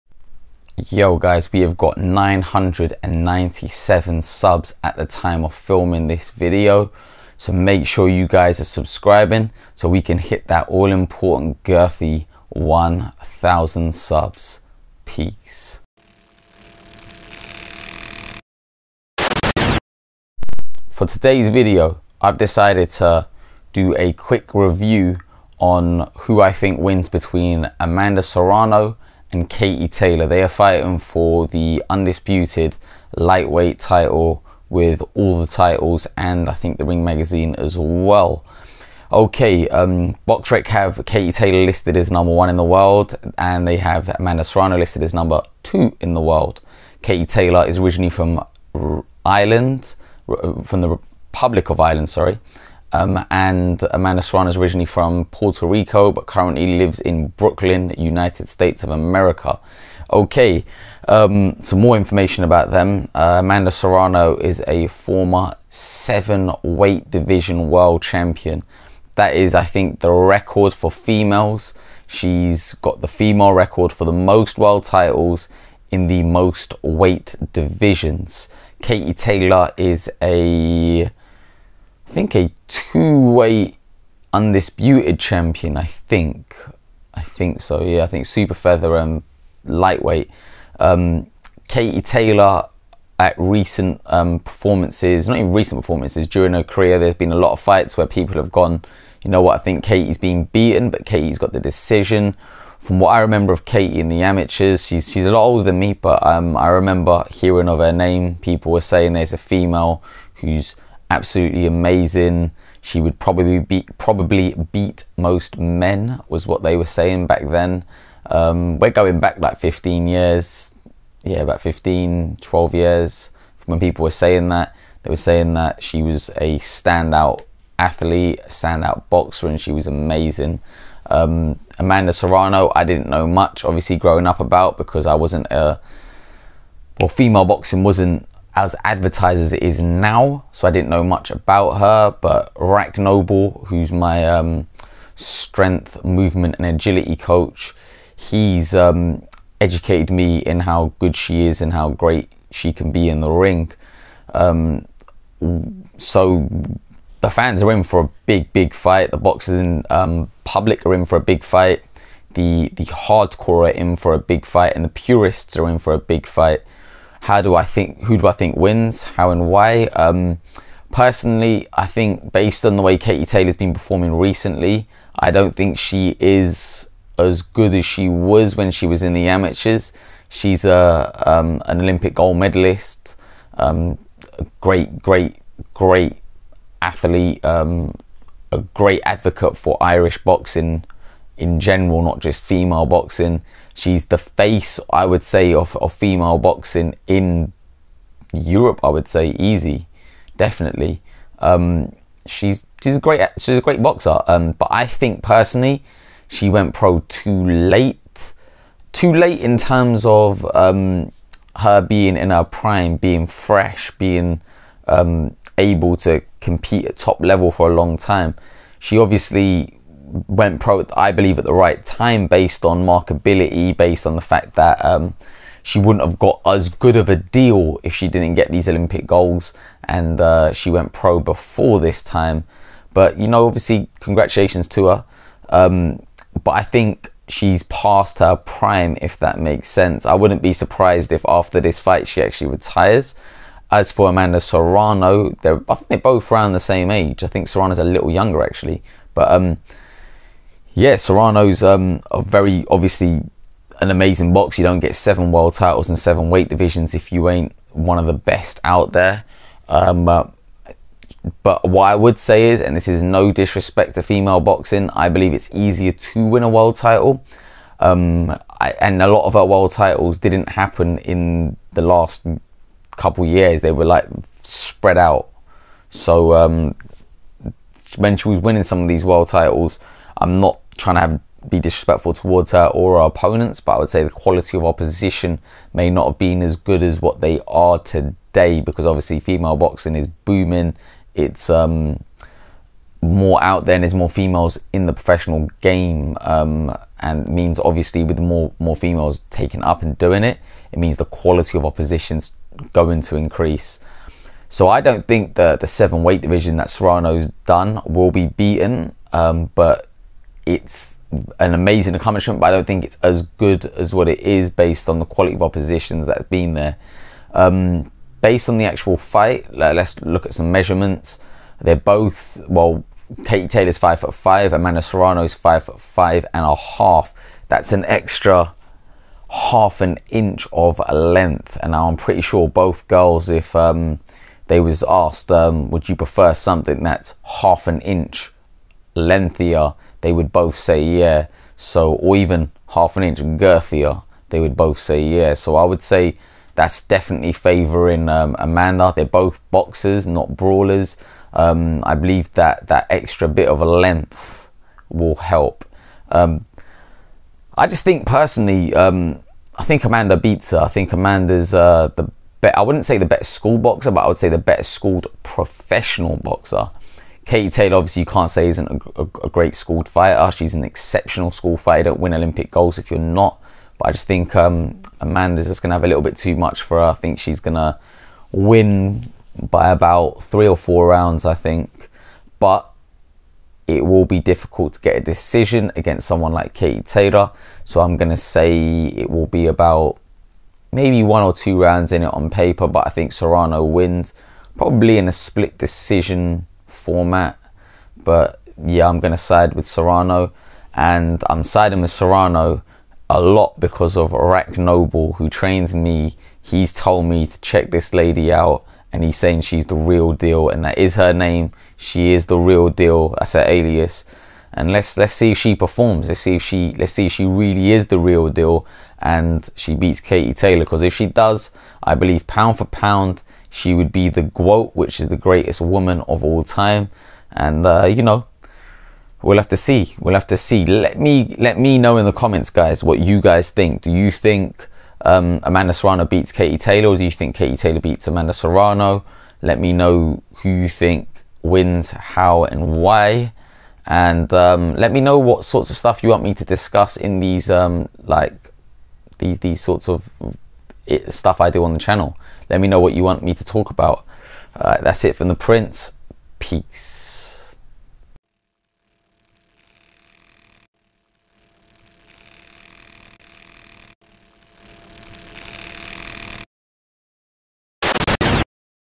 AMANDA SERRANO VS KATIE TAYLOR: IS THIS THE BIGGEST FEMALE FIGHT IN BOXING HISTORY?!
Prince Patel gives a pre-fight review of the Amanda Serrano Vs Katie Taylor match.